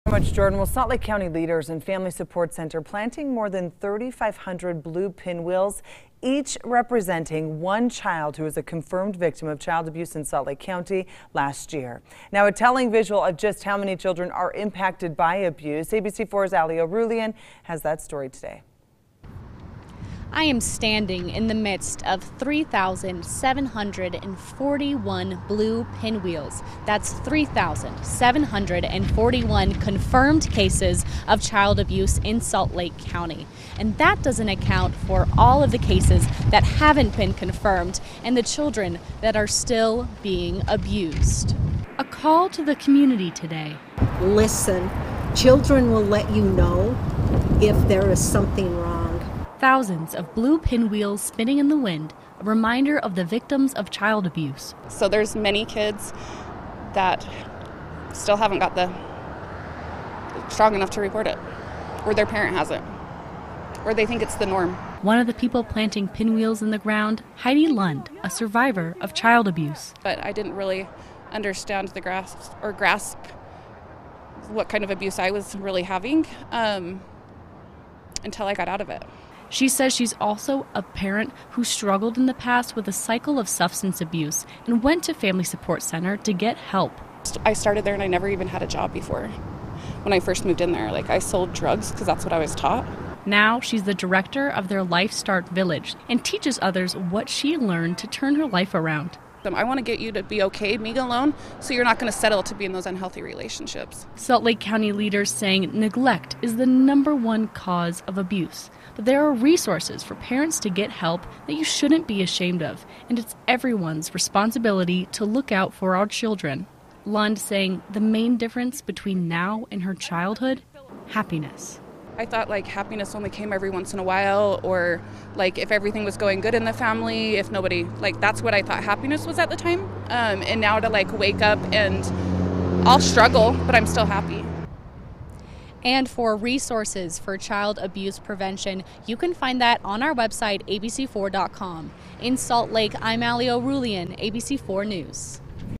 Pinwheels planted, raising awareness of child abuse in Salt Lake County
Salt Lake County leaders and the Family Support Center have planted 3,741 blue pinwheels, each representing one child who's a confirmed victim of child abuse in Salt Lake County last year.